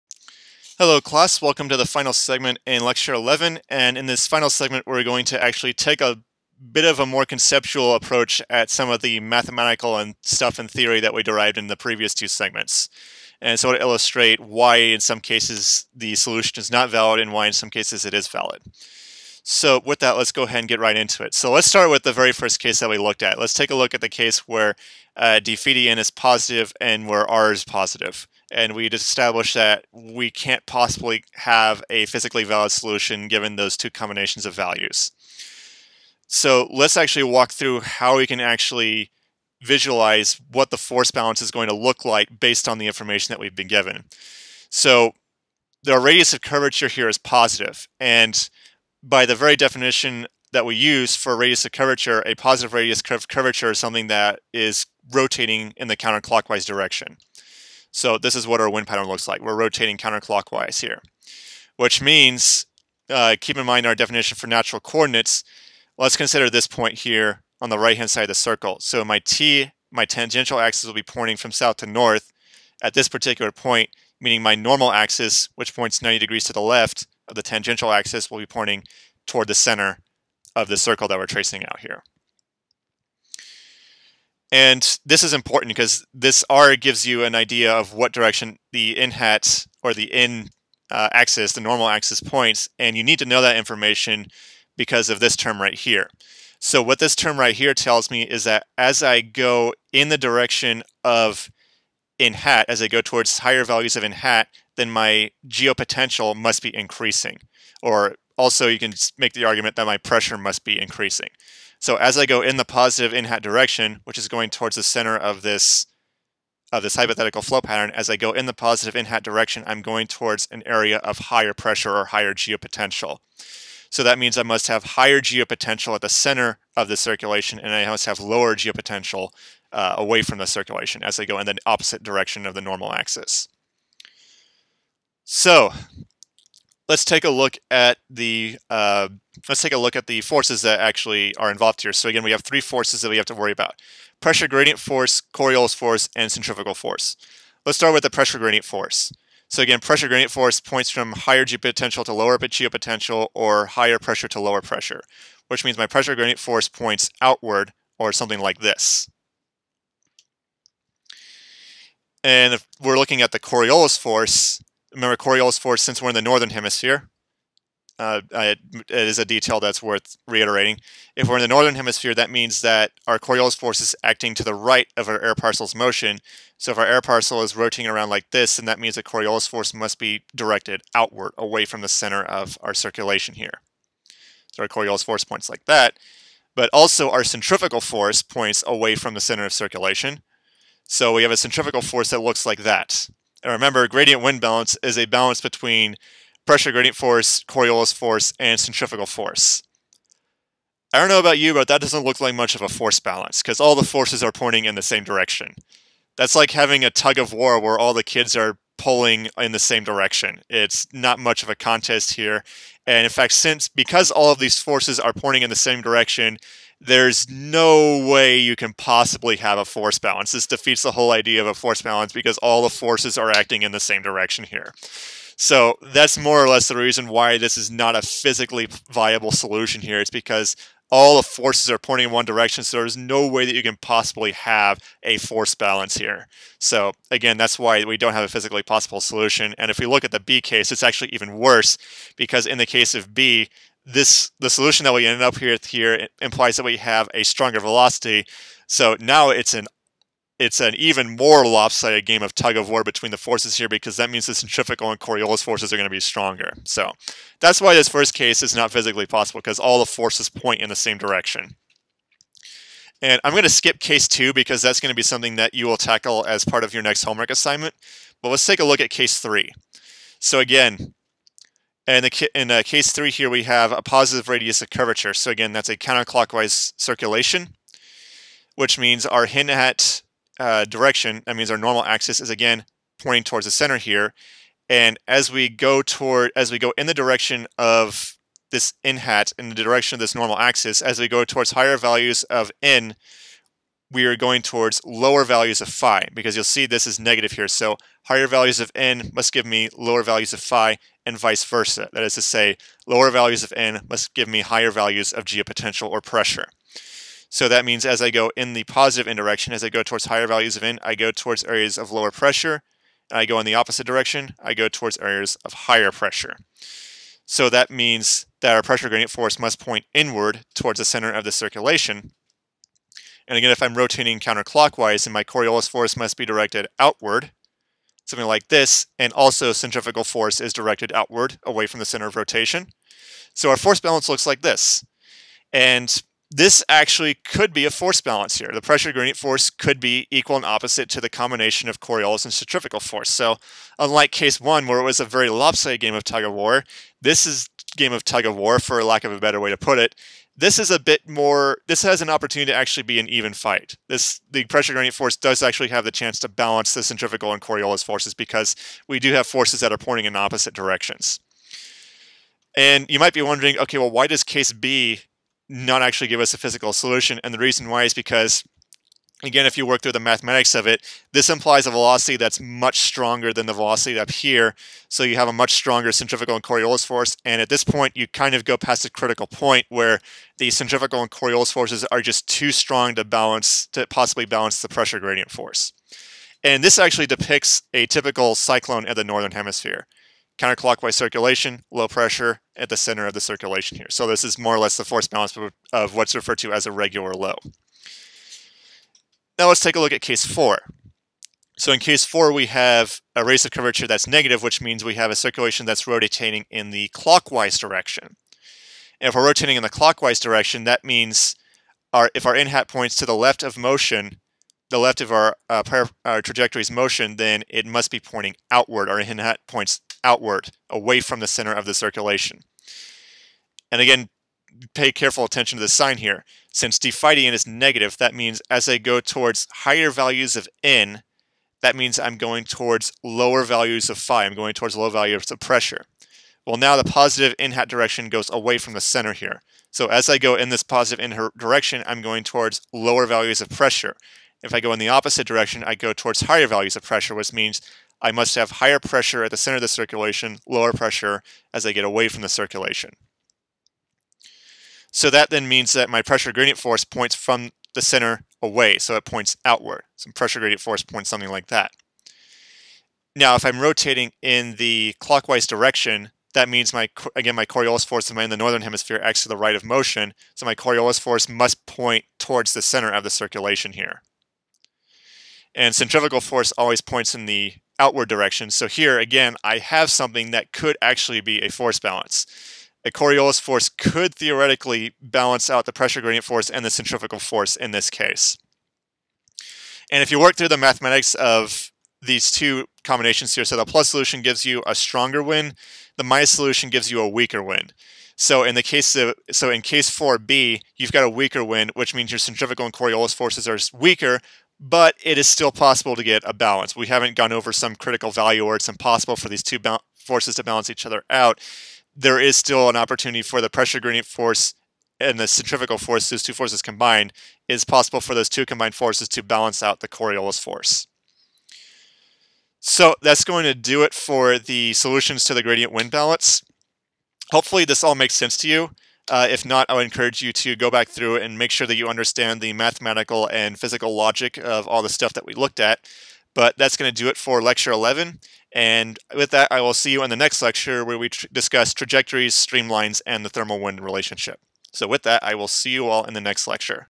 METR2023 - Lecture 11 - Segment 3: Illustrating the Solutions of the Gradient Wind Balance
A graphical representation of what we explored in the previous segment.